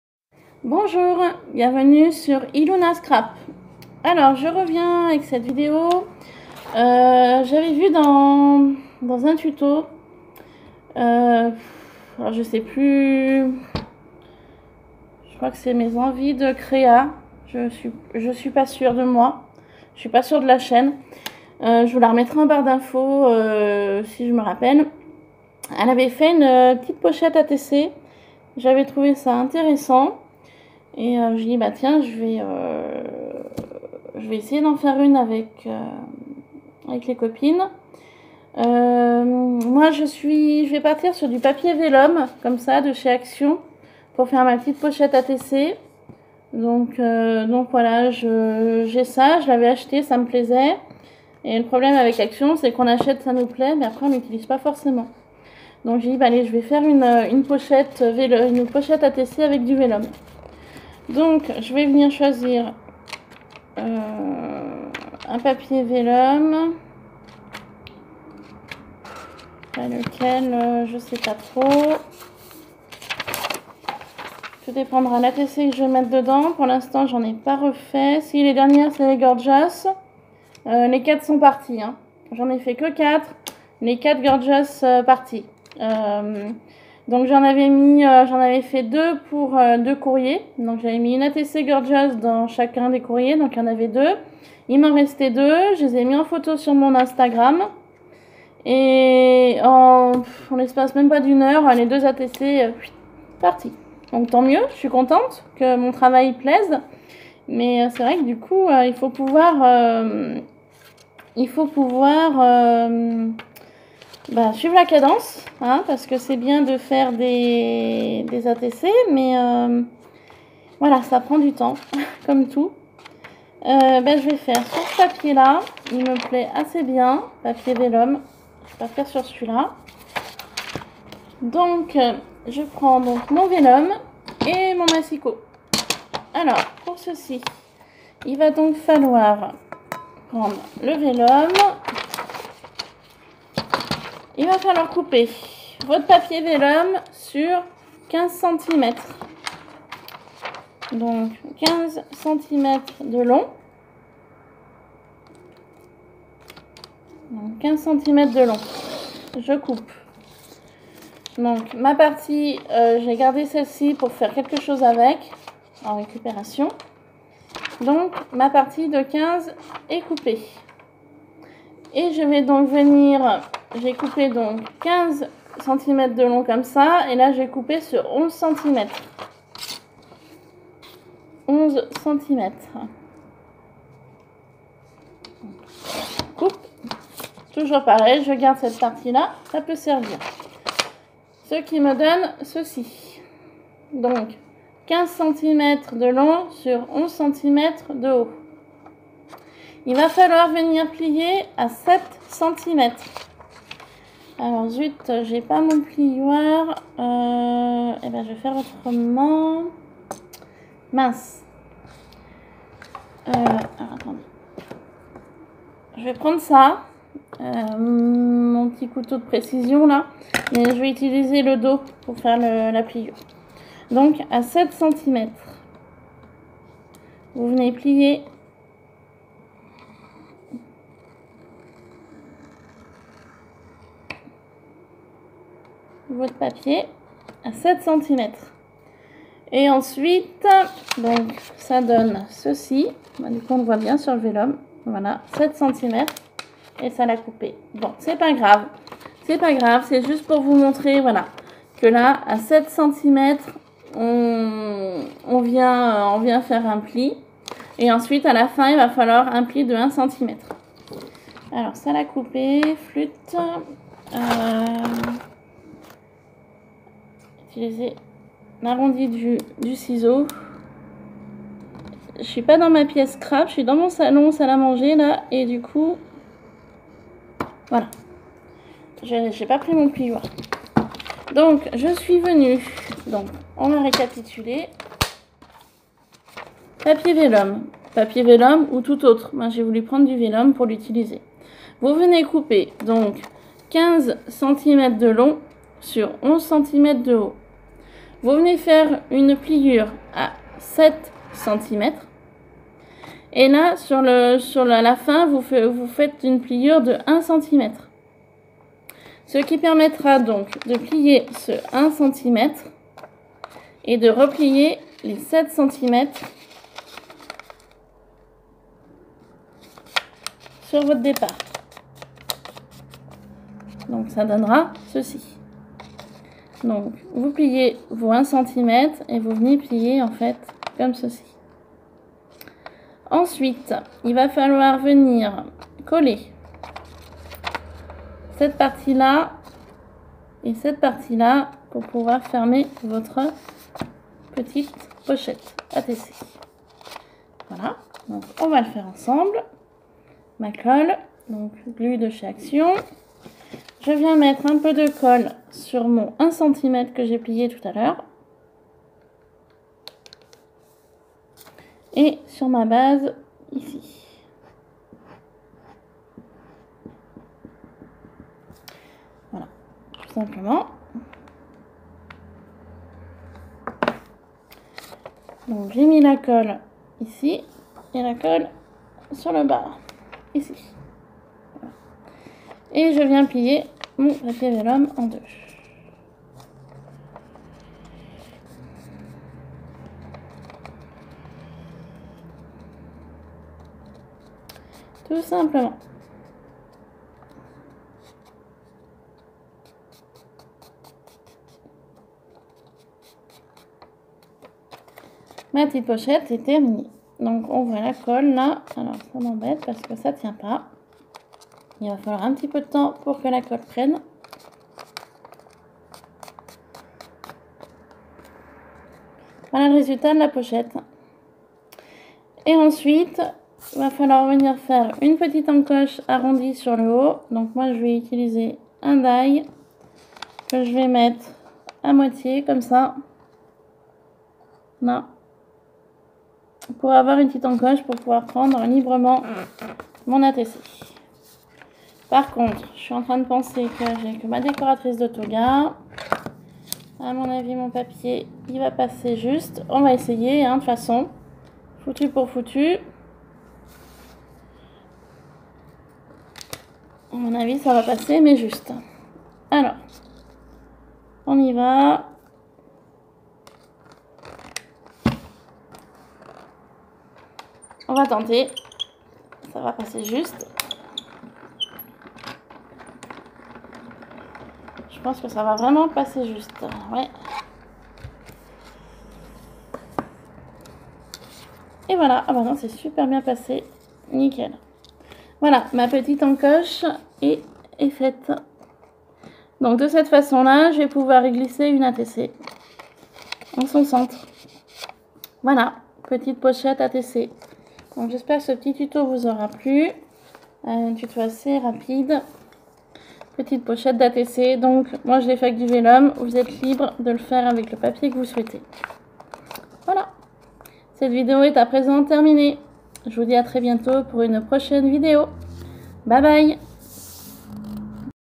Tuto pochette ATC
Utiliser un papier imprimer ou du welom.

Couper selon les dimensions suivante:

15 cm de longueur
11 cm de largeur

Premier plie à 7 cm
Deuxième plie à 14 cm

Il va vous rester 1 cm pour le pliage , pour former la pochette

Plus cas coller et terminer la pochette

Mon instagram: 4314i